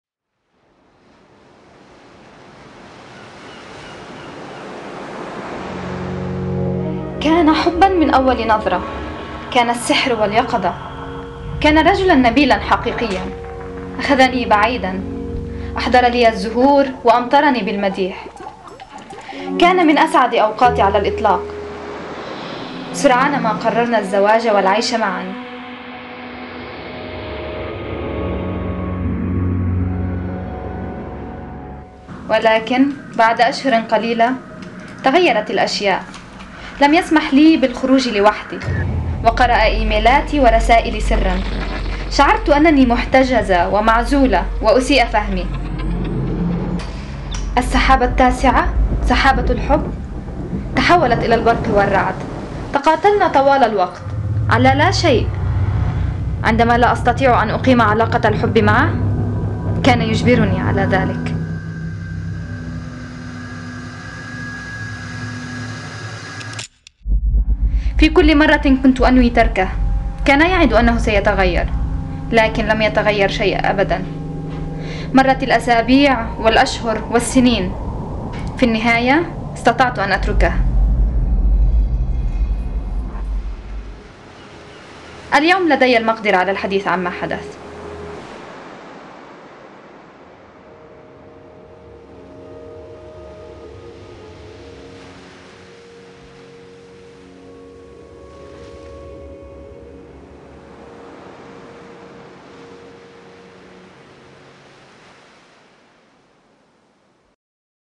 حملة 16 يوم لمناهضة العنف المبني على النوع الاجتماعي في فلسطين
16 days activism against Gender based violence in Palestine

This film was produced for the "16 Days against gender-based violence" campaign by the Swiss NGO Cfd (Feminist Peace Organisation and the Arabic version was done in collaboration with Women and Media in Palestine our partner organisation in Palestine.